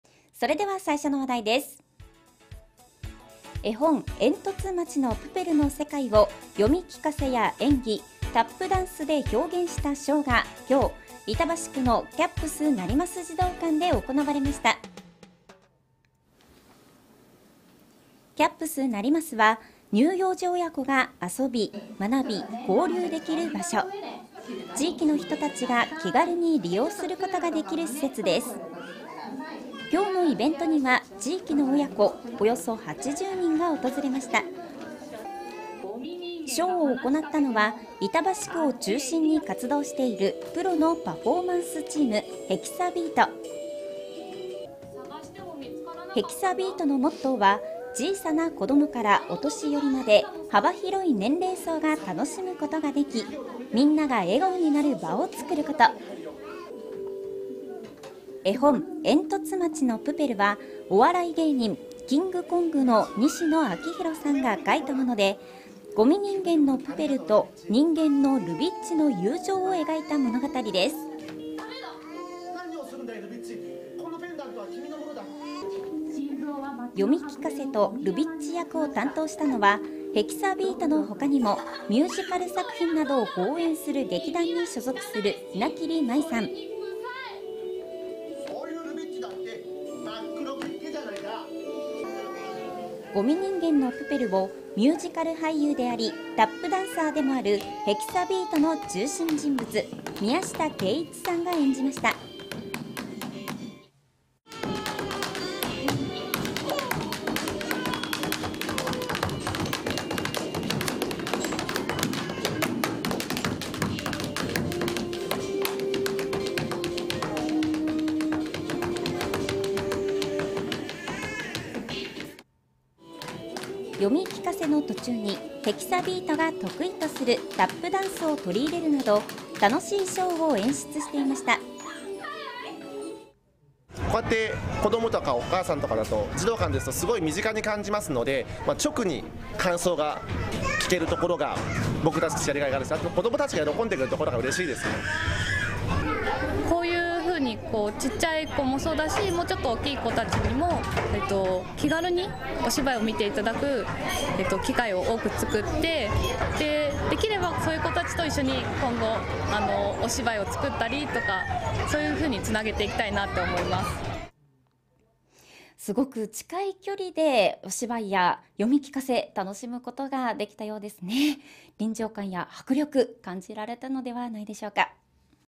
えんとつ町のプペル 読み聞かせとタップダンスショー
2018年1月18日(木)  会場:なります児童館
出演　名桐 舞　宮下 恵一(HexaBeat)

名桐 舞
演劇集団Rubbishに所属

岡山県岡山市出身のミュージカル俳優、タップダンサー
関東近郊で活動を展開しているタップダンスユニット、｢Hexa Beat（ヘキサビート）｣の中心人物
タップダンスのステージやワークショップ活動が中心。活動の一環として、地域福祉施設や介護施設などでも実施

宮下恵一を中心とした、役者やダンサーらによるタップダンスチーム
児童施設・福祉施設・高齢者施設・病院などで利用者の方々に合わせたショーやワークショップを行っている

参考作品：えんとつ町のプペル
作者：西野亮廣（キングコング）